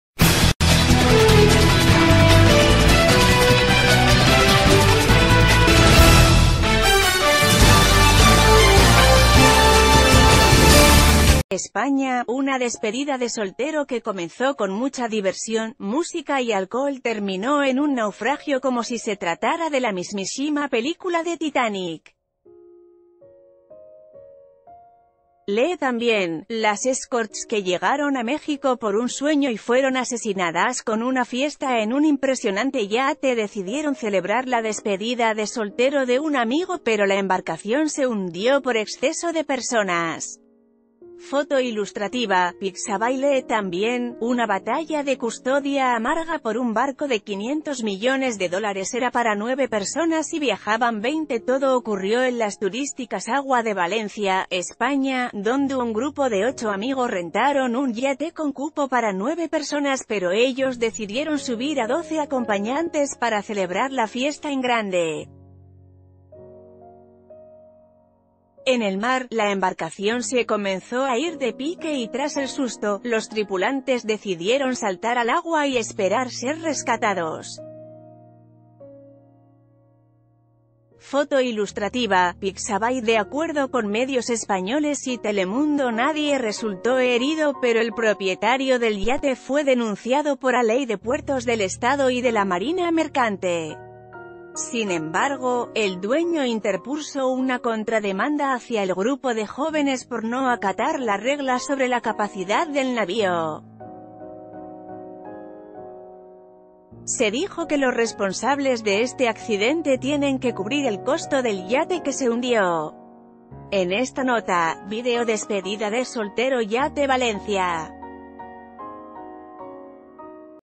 Festejan despedida en un yate y se hunde por exceso de escorts
España.- Una despedida de soltero que comenzó con mucha diversión, música y alcohol terminó en un naufragio como si se tratara de la mismisíma película de Titanic. Lee también: Las escorts que llegaron a México por un sueño y fueron asesinadasCon una fiesta en un impresionante yate decidieron celebr...